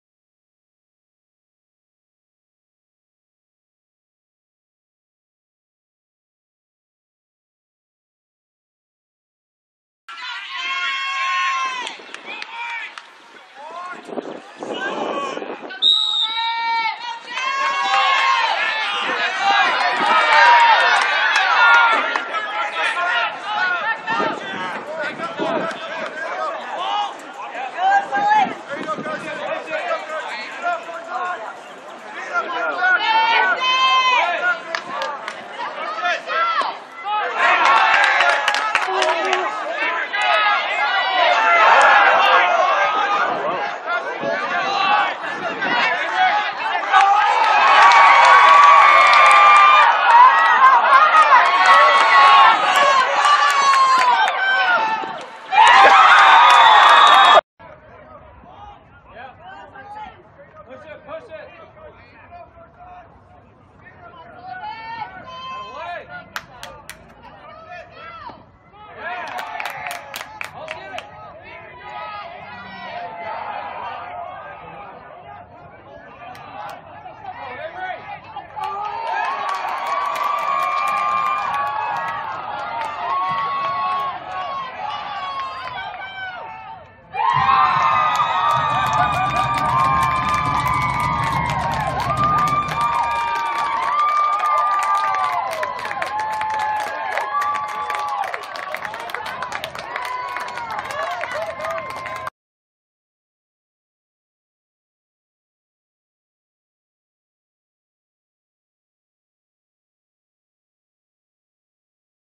Goalie Goal
Brave Heart Goalie Goal. MVs first ever goalie goal! Defense: Carson huerter